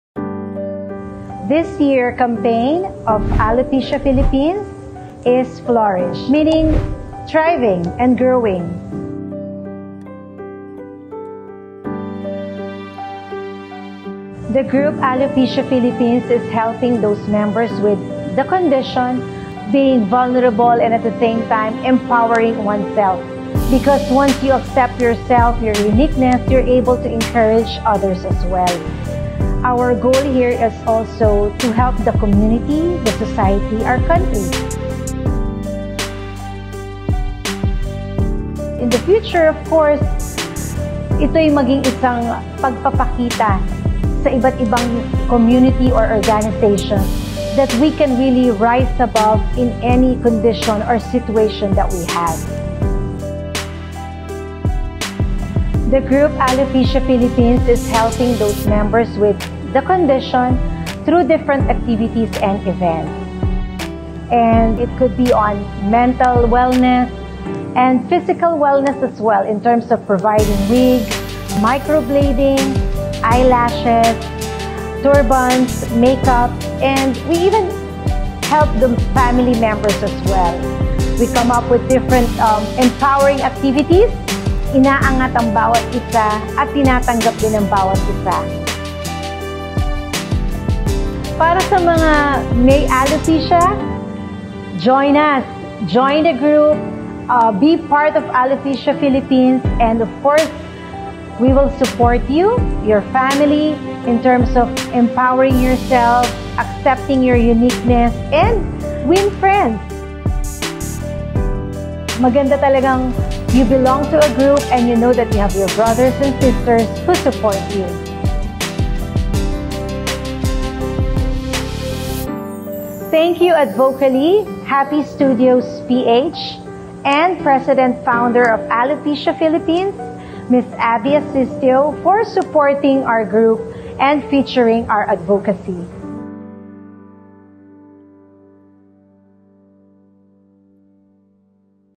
Alopecia Philippines Flourish Video
September is Alopecia Awareness Month, and for 2021 our campaign is Flourish!  Flourishing amidst any condition in life including the pandemic.
Let's continue to encourage and uplift one another in these changing, challenging times!  Blessings to all!